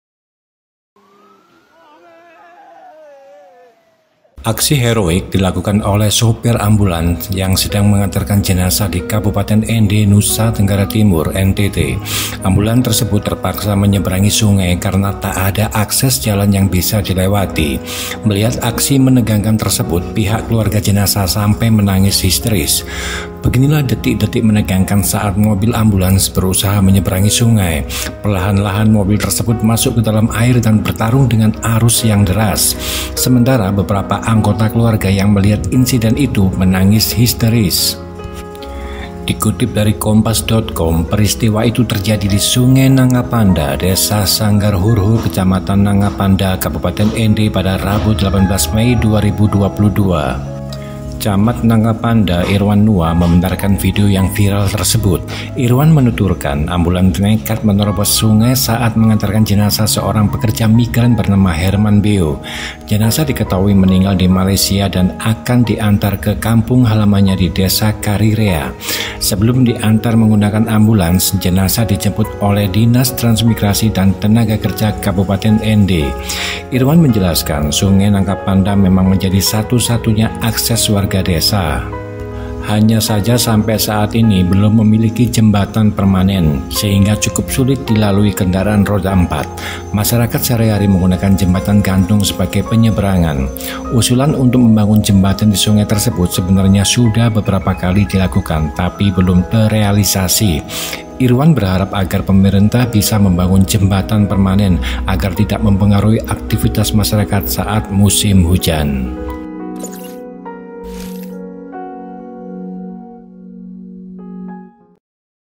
Ambulans Nekat Seberangi Sungai saat Antar Jenazah, Tak Ada Akses Lain yang Bisa Dilewati.
Aksi heroik dilakukan oleh sopir ambulans yang sedang mengantarkan jenazah di Kabupaten Ende, Nusa Tenggara Timur (NTT).

Ambulans tersebut terpaksa menyeberangi sungai, karena tak ada akses jalan yang bisa dilewati.

Melihat aksi menegangkan tersebut, pihak keluarga jenazah sampai menangis histeris.

Beginilah detik-detik menegangkan saat mobil ambulans berusaha menyeberangi sungai.

Perlahan-lahan mobil tersebut masuk ke dalam air dan bertarung dengan arus yang deras.

Sementara beberapa anggota keluarga yang melihat insiden itu menangis histeris.

Dikutip dari Kompas.com, peristiwa itu terjadi di Sungai Nangapanda, Desa Sanggarhorho, Kecamatan Nangapanda, Kabupaten Ende pada Rabu (18/5).

Camat Nangapanda, Irwan Nua membenarkan video yang viral tersebut.

Irwan menuturkan, ambulans nekat menerobos sungai saat mengantar jenazah seorang pekerja migran bernama Herman Beo.

Jenazah diketahui meninggal di Malaysia dan akan diantar ke kampung halamannya di Desa Karirea.

Sebelum diantar menggunakan ambulans, jenazah dijemput oleh Dinas Transmigrasi dan Tenaga Kerja Kabupaten Ende.

Irwan menjelaskan sungai Nangapanda memang menjadi satu-satunya akses warga desa.

Hanya saja sampai saat ini belum memiliki jembatan permanen, sehingga cukup sulit dilalui kendaraan roda empat.

Masyarakat sehari-hari menggunakan jembatan gantung sebagai penyeberangan.

Usulan untuk membangun jembatan di sungai tersebut sebenarnya sudah beberapa kali dilakukan, tapi belum terealisasi.

Irwan berharap agar pemerintah bisa membangun jembatan permanen, agar tidak mempengaruhi aktivitas masyarakat saat musim hujan.

(ren)